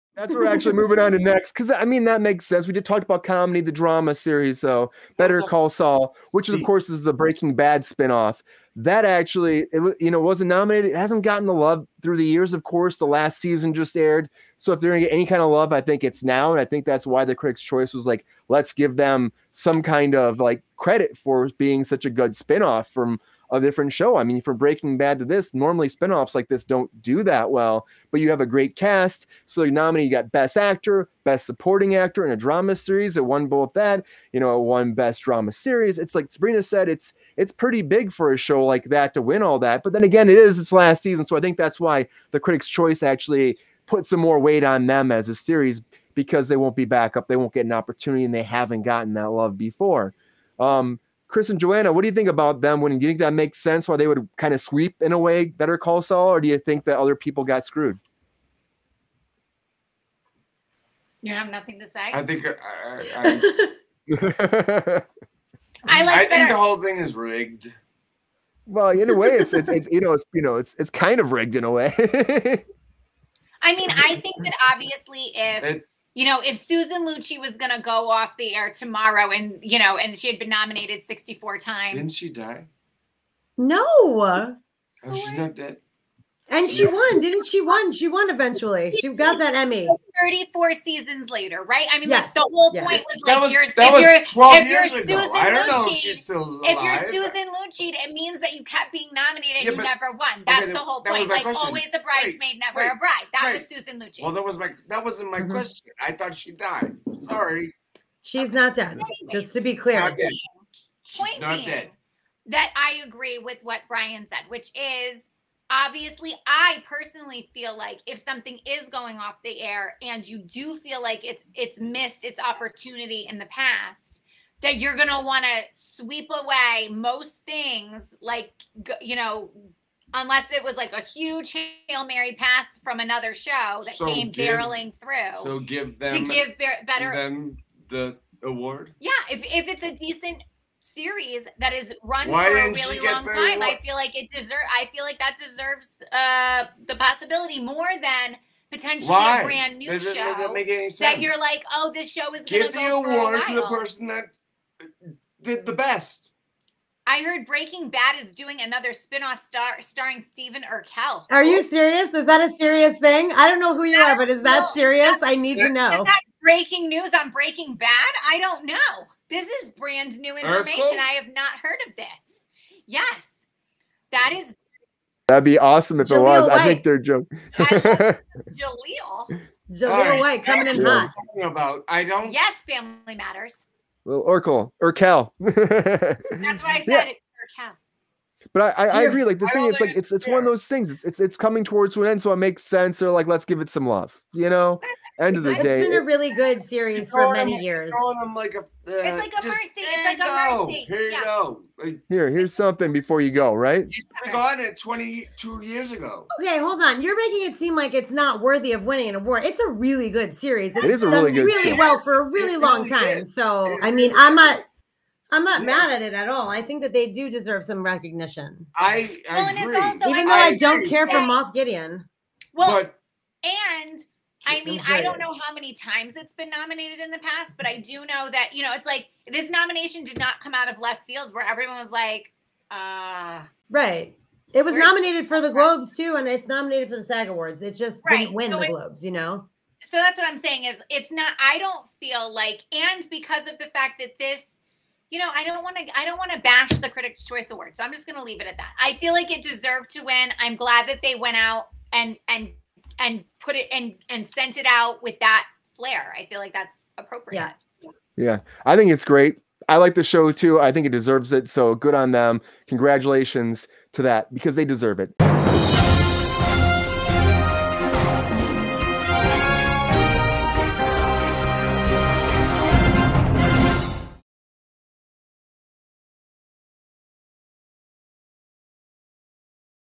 Better Call Saul WINS Best DRAMA SERIES!
We discuss Better Call Saul winning Best Drama Series at the Critics Choice Awards.